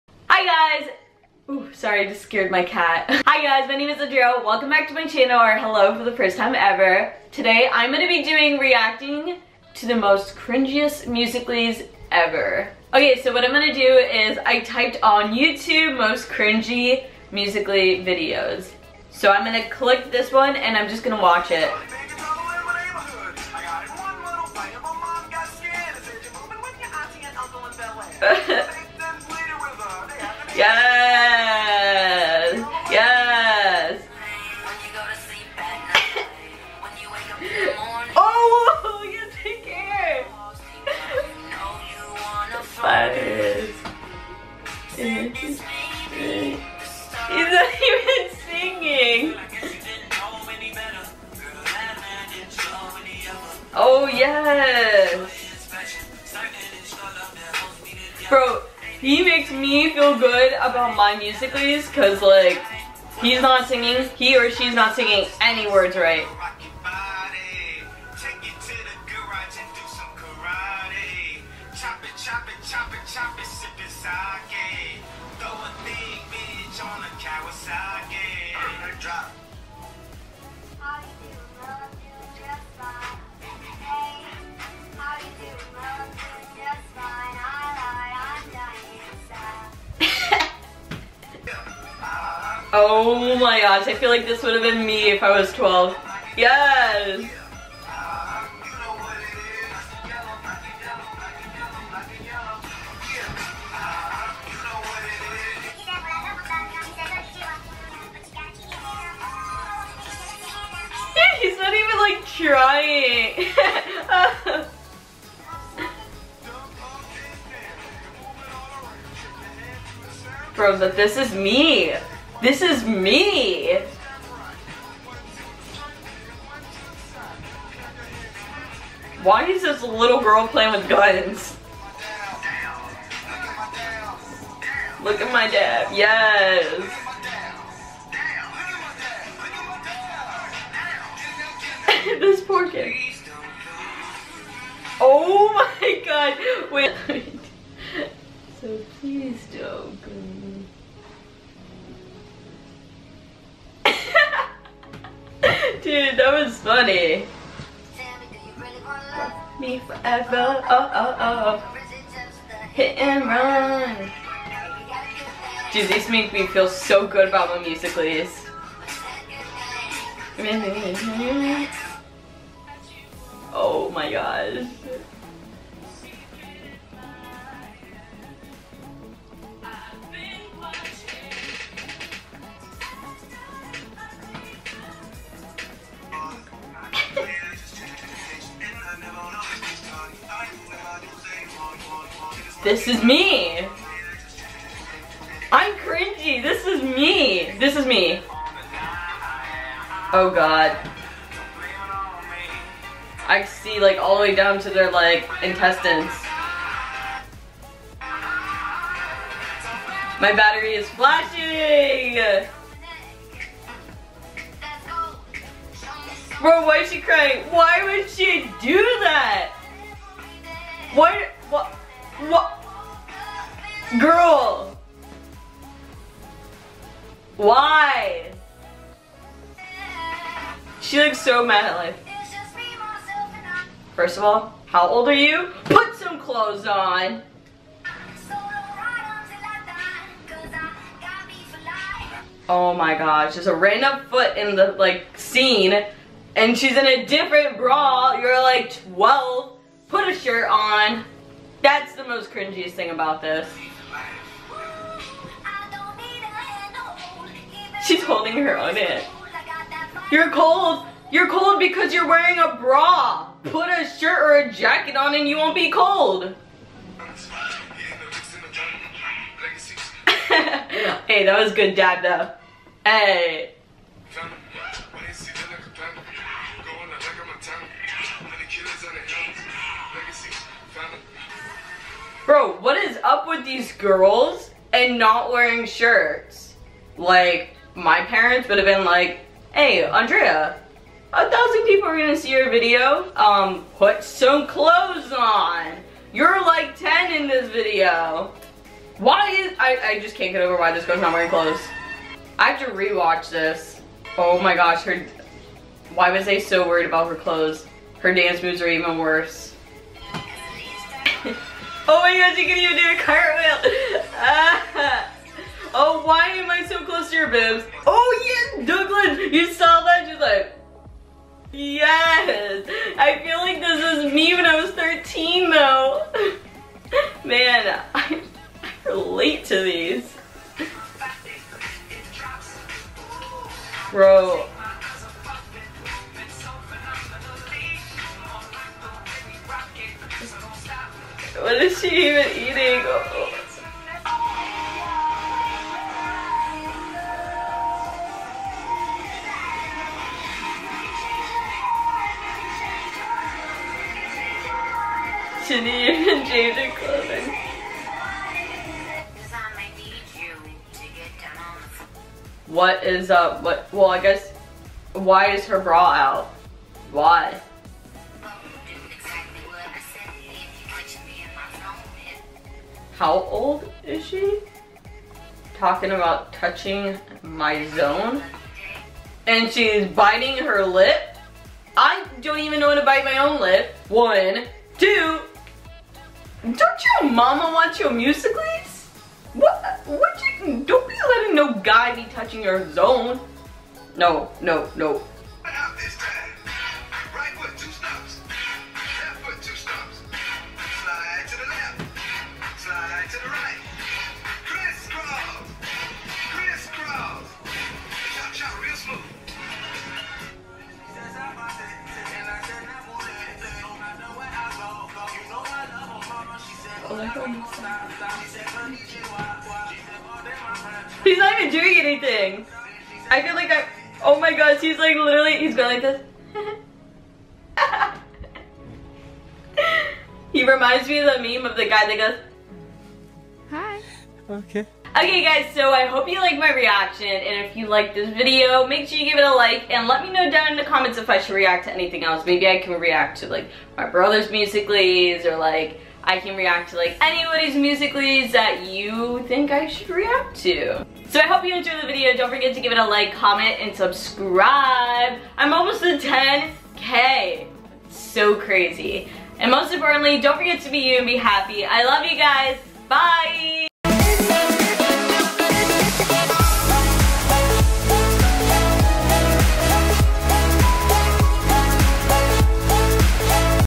REACTING TO CRINGY MUSICAL.LYS | ANDREA M JARA
Hi!!! Hope you enjoy and Let me know if you want me to do a different reaction video!

DO NOT FORGET TO BE YOU AND BE HAPPY!